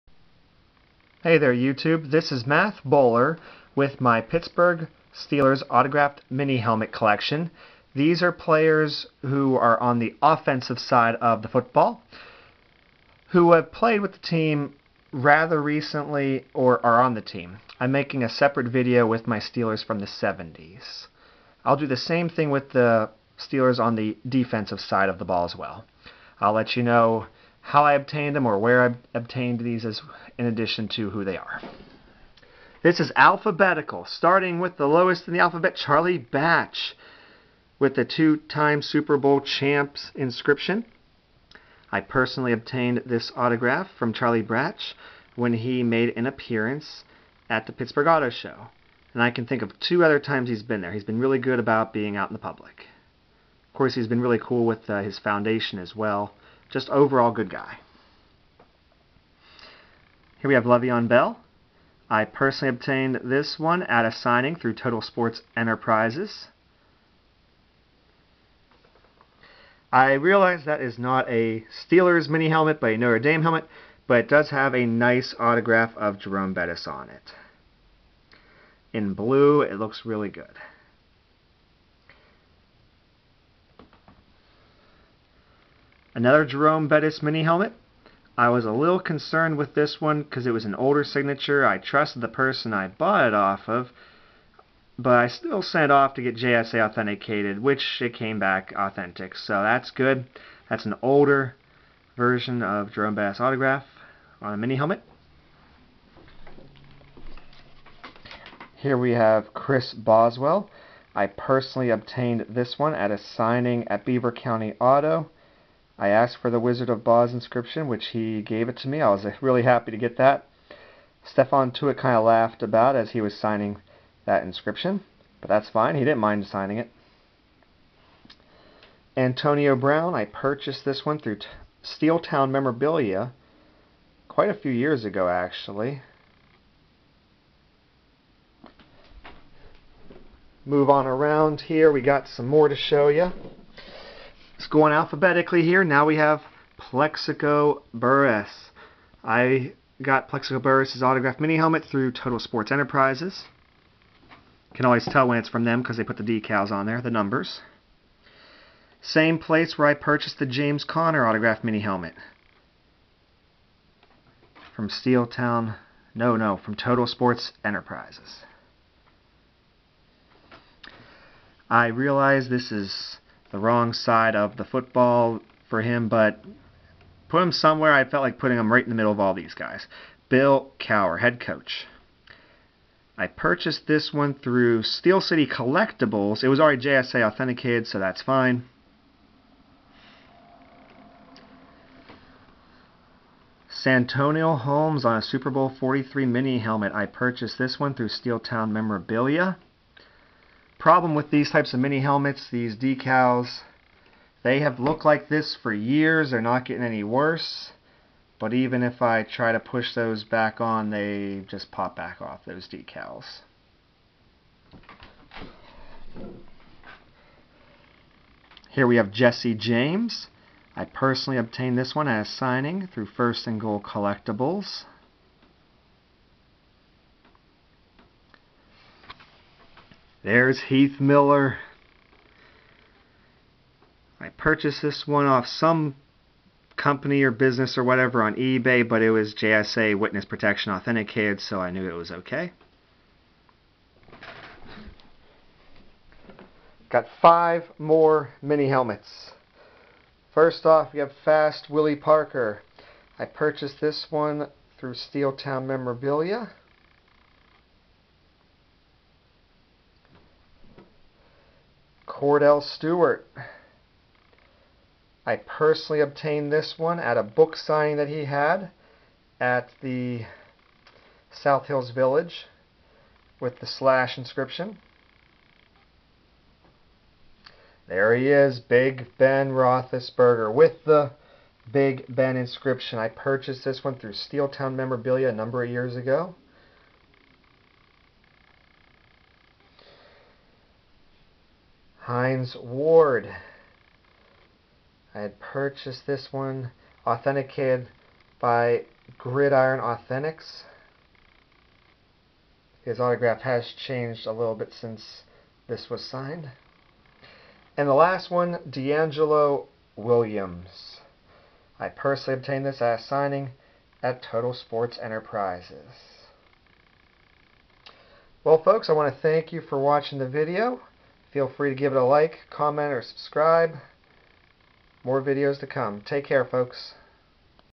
Steelers Autograph Mini Helmet Collection of Current or Recent Offensive Players
This videos shows my autographed Steeler mini helmets of offensive players of current or recent players.